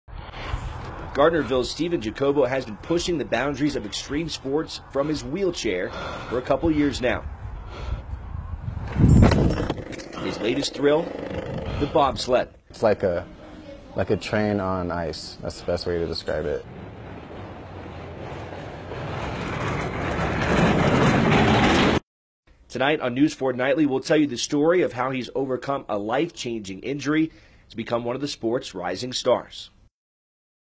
Steven Jacobo pushing boundaries
Intro to my latest news report from Reno News 4. They been keeping up with me the last 2-3 years following my achievements. You can also find me on -

                   Or
                   Steven Jacobo-ParaBobsledder